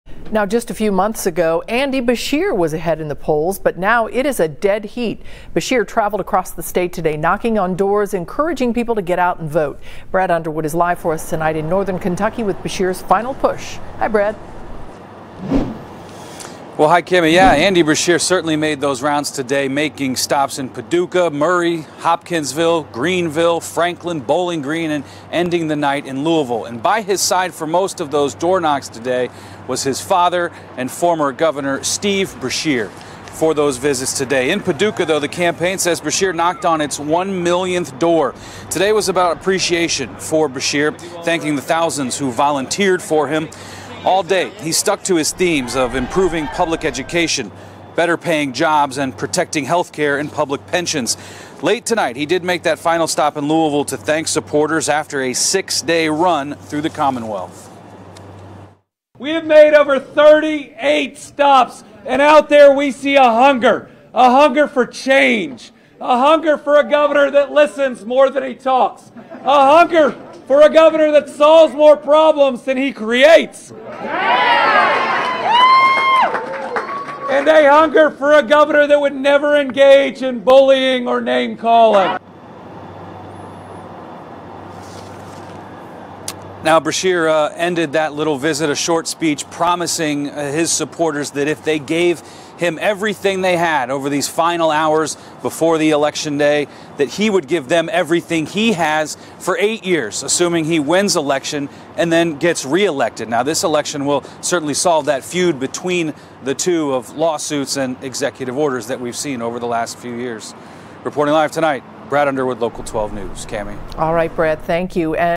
Beshear spends Election Day eve knocking on doors, thanking supporters throughout Kentucky
LOUISVILLE, Ky. (WKRC) - Kentucky’s Attorney General Andy Beshear spent Election Day eve on the road knocking on doors in Paducah, Murray, Hopkinsville, Franklin, Greenville and Bowling Green. His campaign says that in Paducah, the 1 millionth door was knocked on.

With his dad, former governor Steve Beshear by his side, the gubernatorial candidate thanked supporters.

“Our Western Kentucky values say that we care out each and every family in Kentucky,” said Beshear. “I’m going to be a governor for each one of them.“

All day, Beshear stuck to his campaign themes of improving public education, better paying jobs, protecting health care and public pensions.

“The future of public education is on the line. The future of rural health care is on the line. Whether we demand decency out of the governor’s office is on the line,” said Besher. “I believe voters are ready. They are going to send a message that Matt Bevin and his behavior is the exception and not the rule as we are going to get back to being the good Kentuckians we were raised to be. “

Beshear finished the night in Louisville at an appreciation event for volunteers and supporters.